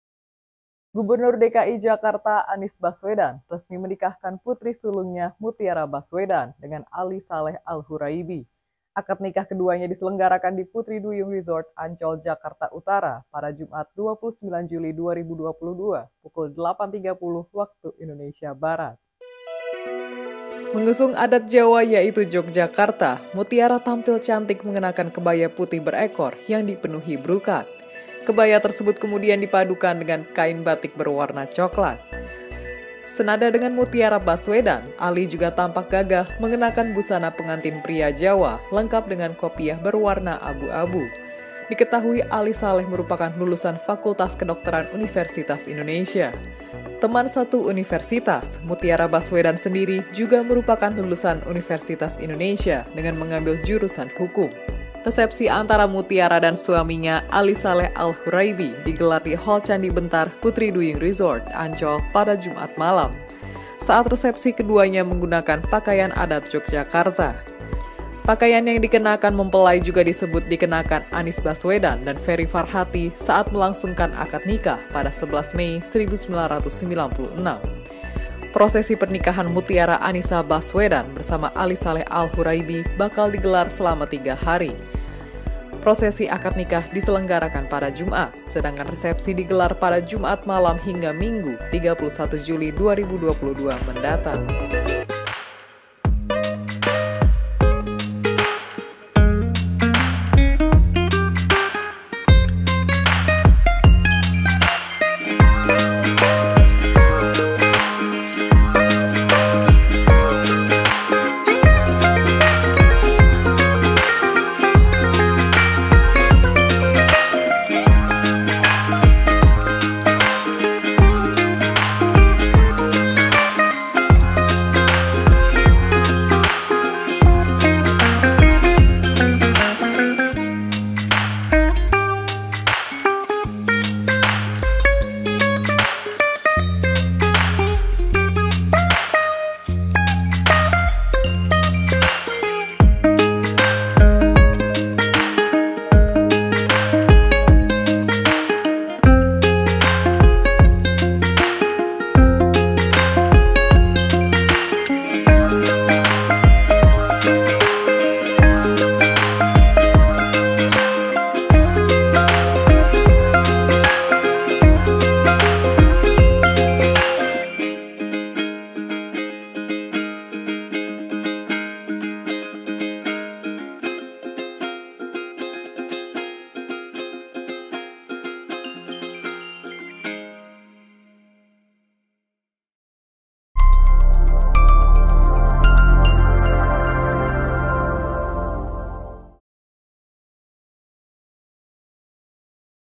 Prosesi Pernikahan Putri Anies Baswedan Pakai Adat Jawa Jogja
Gubernur DKI Jakarta, Anies Baswedan resmi menikahkan putri sulungnya, Mutiara Baswedan dengan Ali Saleh Alhuraiby.
Akad nikah keduanya diselenggarakan di Putri Duyung Resort, Ancol, Jakarta Utara pada Jumat (29/7/22) pukul 08.30 WIB.
Mengusung adat Jawa, yaitu Yogyakarta, Mutiara tampil cantik mengenakan kebaya putih berekor yang dipenuhi brukat.